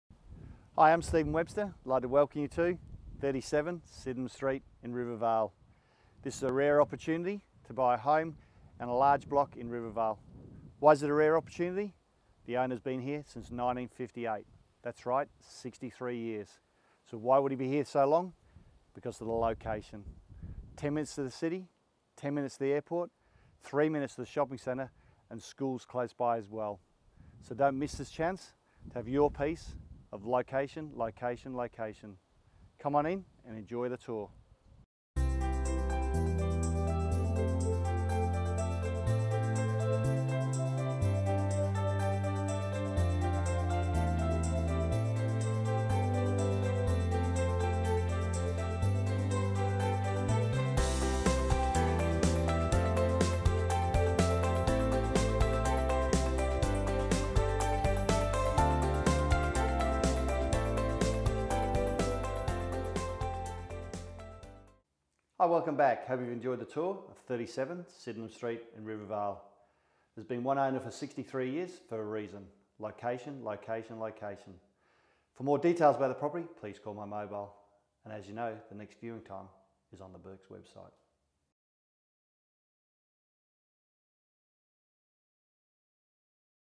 37 Sydenham Street, Rivervale, Western Australia
RIVERVALE RENOVATION
I'm old and I need a new lover!!

Built in 1958 the original owner has decided it's time to move on out and hand this spacious character home over to a new owner.

Situated on a 782 m² block within walking distance to Peet Park, this property is worth viewing immediately as it will sell quickly.

Comprising four bedrooms and one bathroom with lounge and dining areas plus a renovated kitchen and rear laundry. The backyard has a large handyman shed, with a lockup garage with stunning trees ready for that special kid's cubby house.

Conveniently located only 7kms from the Perth CBD and on your doorstep, you are greeted with access to the Optus Stadium, and Crown Perth Entertainment Precinct, Swan River, East Perth, Gloucester Park.

A short drive from key facilities including the Belmont Forum, Belmont Oasis Swimming Complex, Reading Cinemas, Domestic and International Airports, CBD, Ascot and Belmont Racecourses, and Mineral Resource Park.